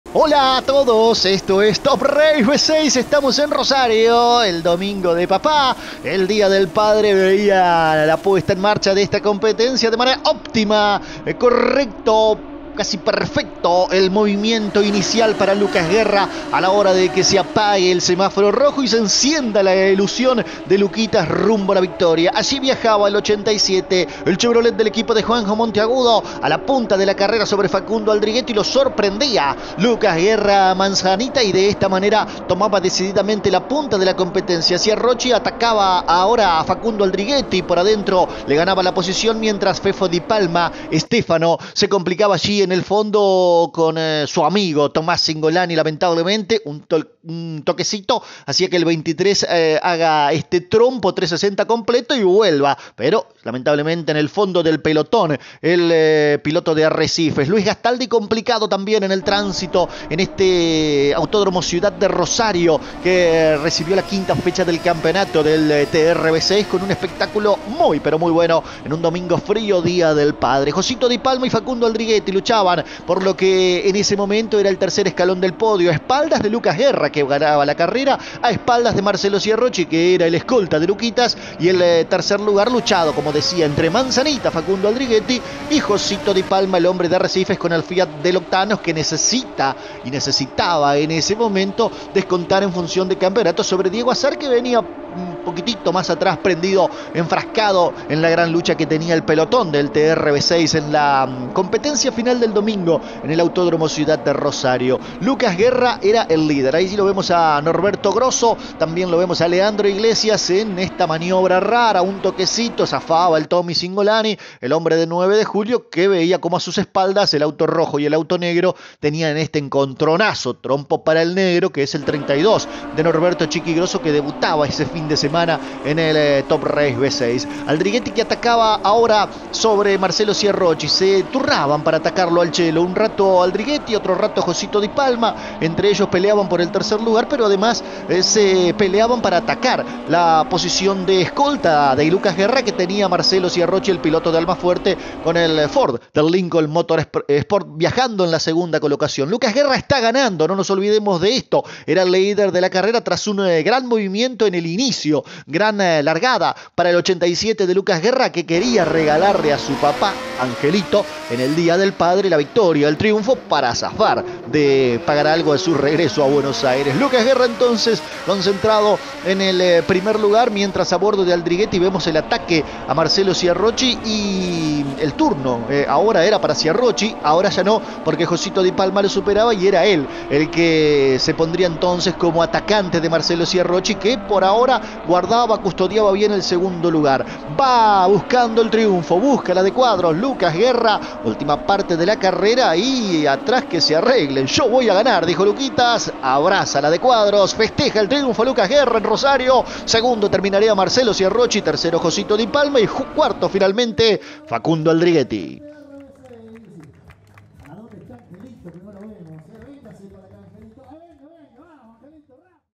TopRace | Autódromo Ciudad de Rosario | Resumen | Final TRV6
El Top Race y Top Race Series se presentan  por la 5ta fecha de sus Campeonatos 2023 y apertura del CARX  en el Autódromo de Rosario.
Relatos en vivo de todas la actividades del fin de semana en la voz de Ale Lezcano.

Para ver más videos de Top Race, visita toprace.com.ar